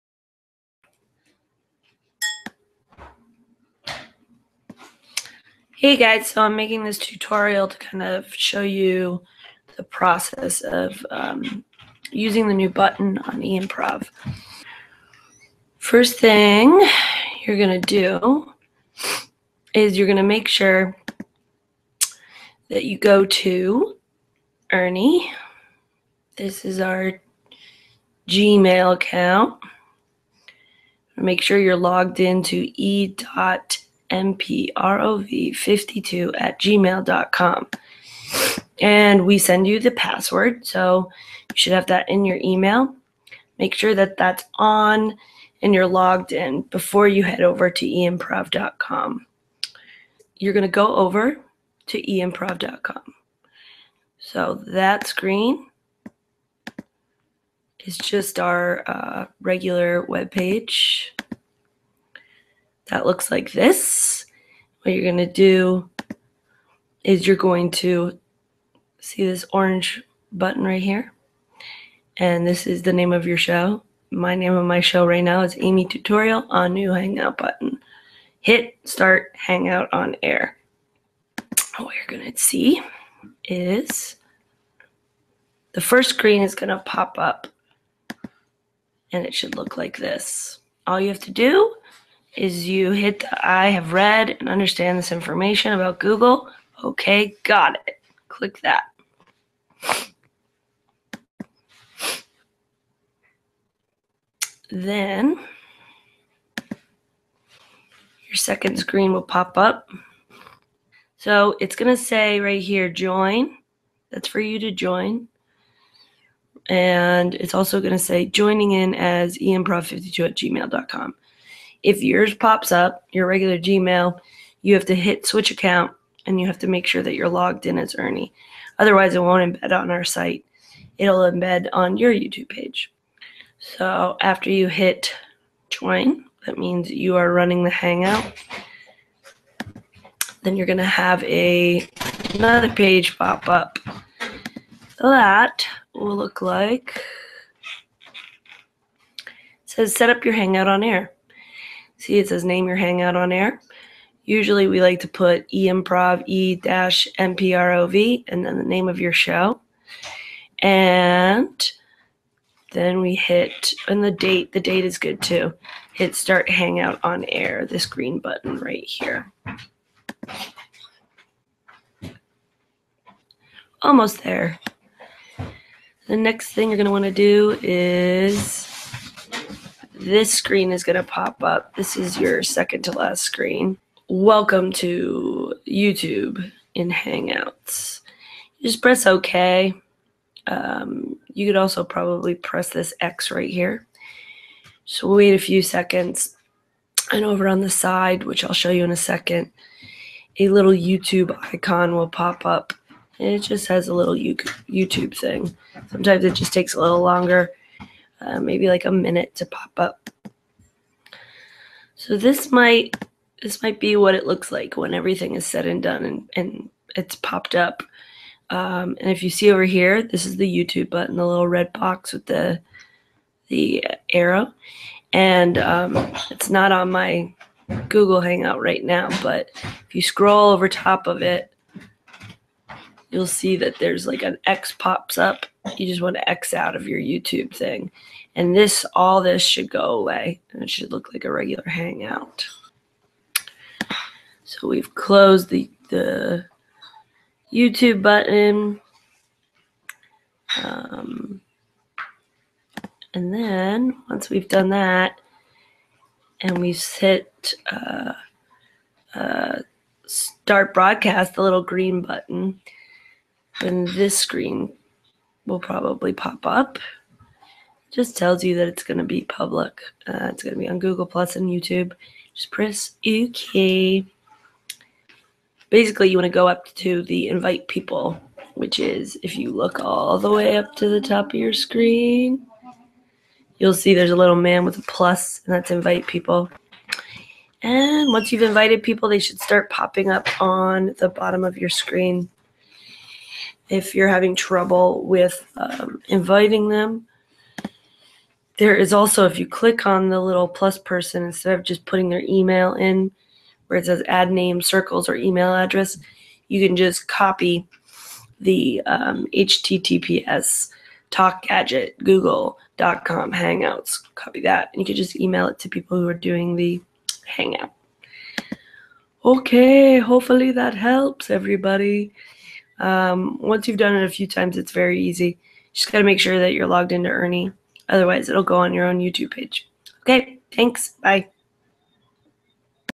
Tutorial on the new button start hangout
guys if you are running into trouble watch this it goes through step by step on how this new button works.